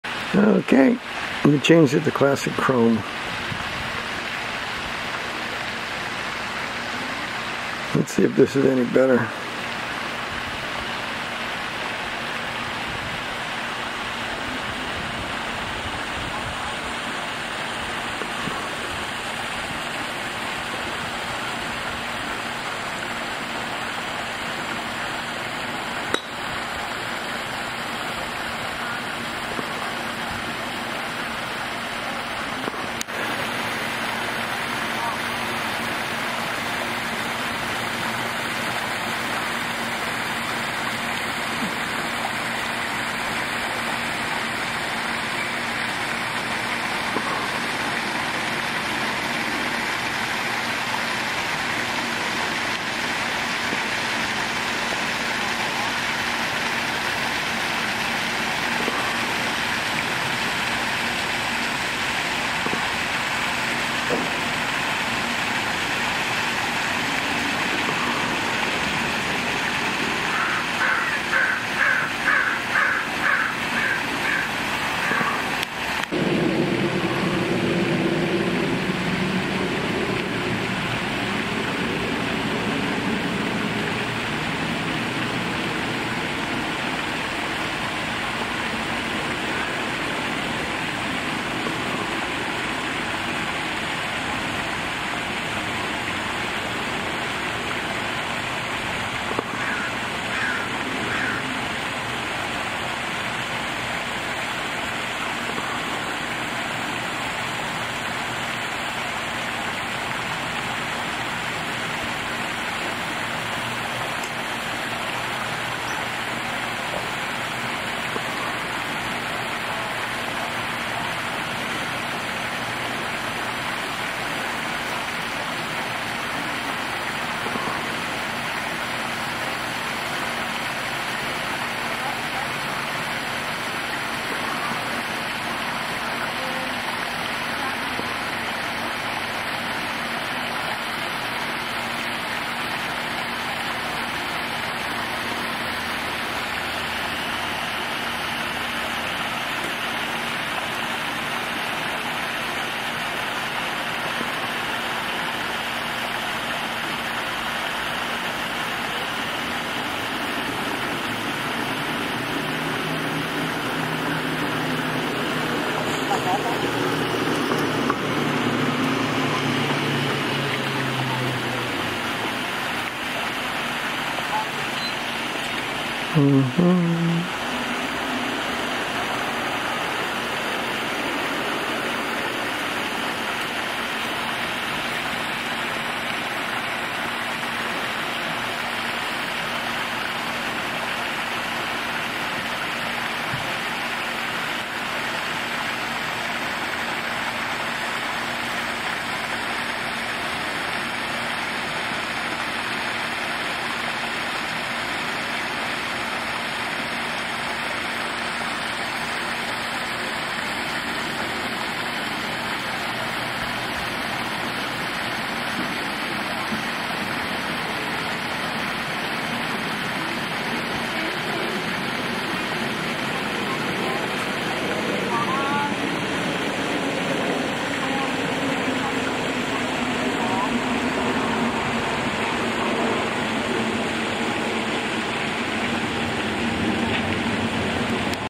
Travel with David -- Test Fuji Classic Chrome film Simulation
Video created by Dave Morton. Filmed using a Samsung Galaxy S20, DJI Action Camera or Fujifilm S-X10